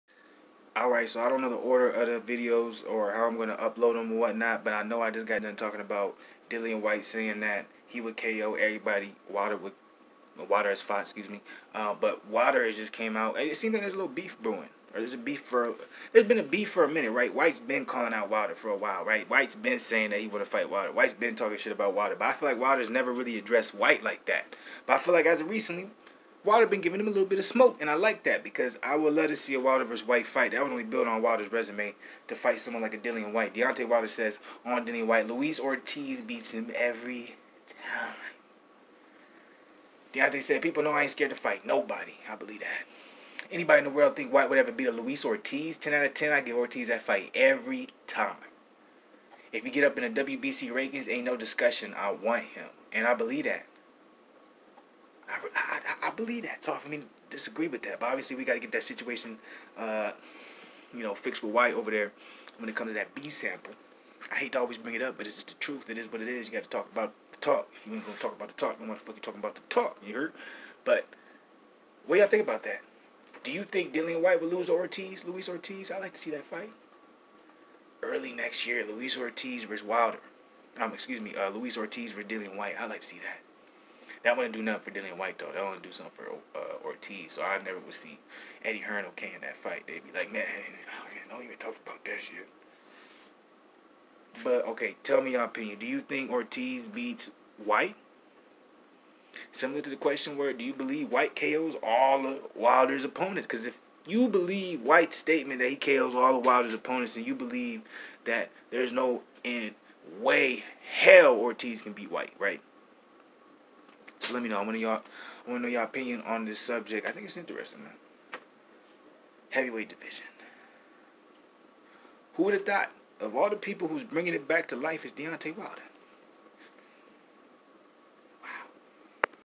DEONTAY WILDER SAYS LUIS ORTIZ GIVES DILLIAN WHYTE WORK IF THEY EVER FOUGHT! WHYTE VS. ORTIZ 2020?
BLAKGOOSETV